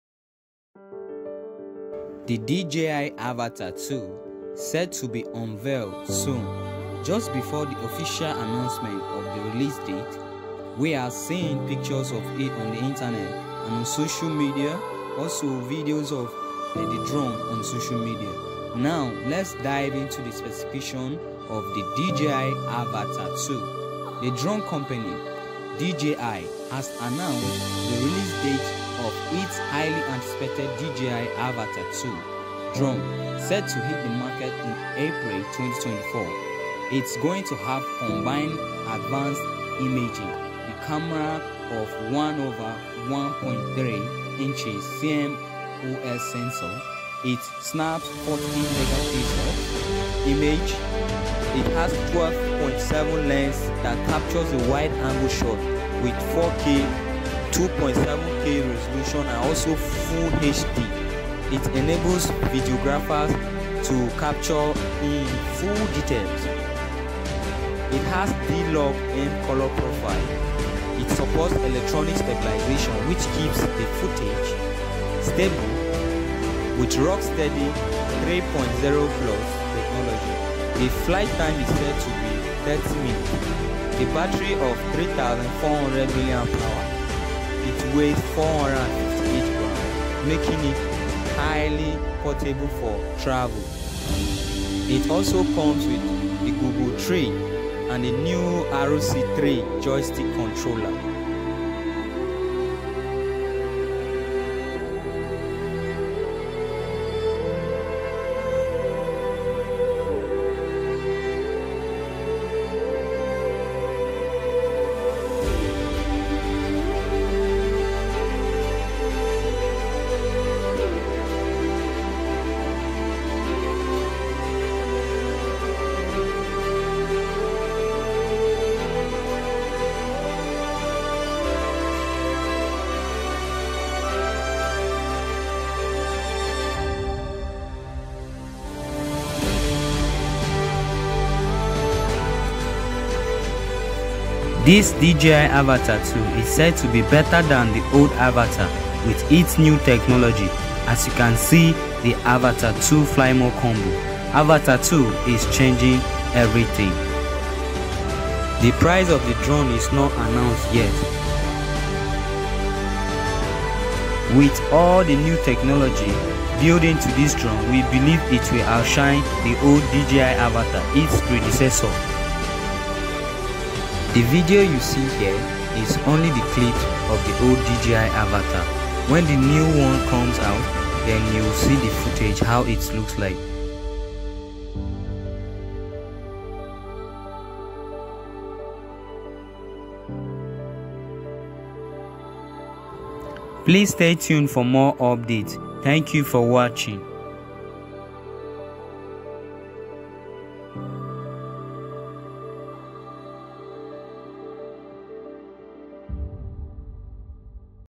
DJI AVATA 2 LEAKED | SPECS | DESIGN | RELEASED and EXPECTATIONS
This is a leaked Video of DJI AVATA 2 Drone, showing the design,specifications,released date and expectations.
Camera:1\1.3inch CMOS sensor, Shoots 4k,2.7k and Full HD Resolutions
Flight Time: 30 Minutes
Battery: 3400Amh
Weight: 458GRM
Released Date: 11 April 2024